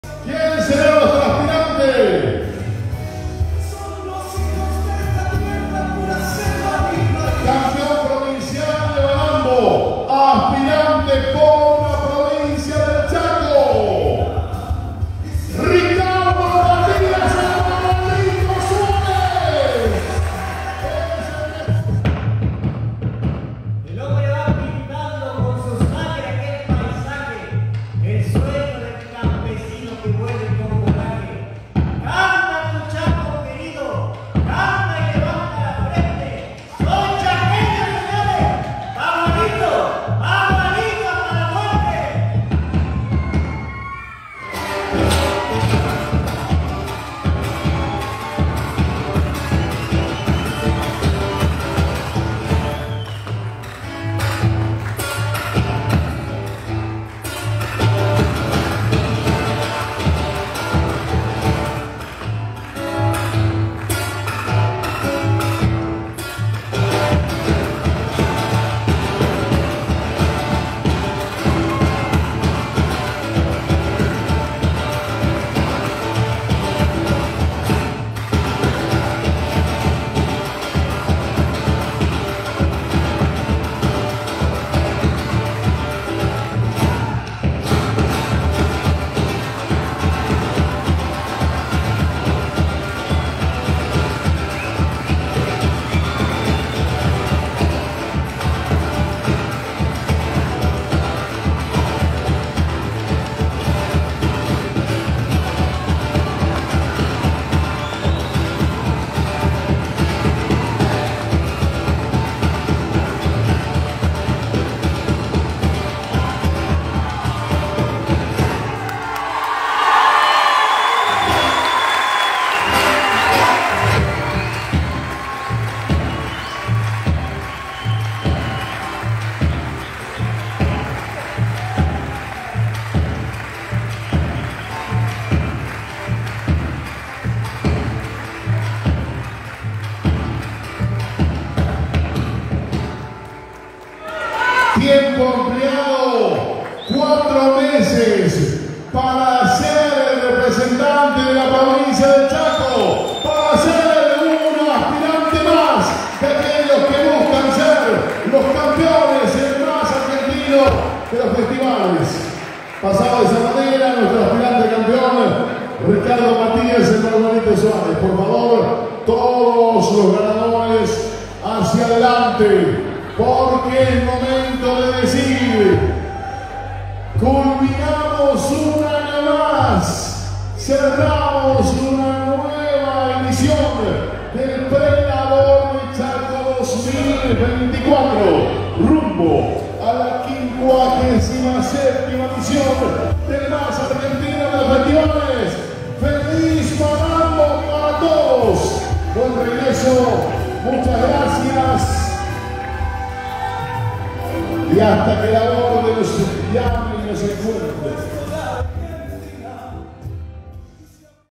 Ricardo Suarez Aspirante a Campeon de Malambo Provincia de Chaco
Charata tiene talentos que siempre se destacan a lo largo de la historia
Ricardo Suárez "Campeón Provincial de Malambo"

Ricardo, hace muchos años que se dedica entre otras cosas, a su carrera artística. Junto a su familia han conformado el grupo de baile "Los Bagualitos" que han recorrido diferentes festivales y escenarios a lo largo de nuestro país y también, en otros países.

El fin de semana pasado se consagró CAMPEÓN PROVINCIAL DE MALAMBO obteniendo así el pase al Festival Nacional de Malambo como ASPIRANTE A CAMPEÓN NACIONAL DE MALAMBO 2025 representando así a la provincia del Chaco y a nuestra querida Perla del Oeste.

Gracias Malambo
video pre laborde chaco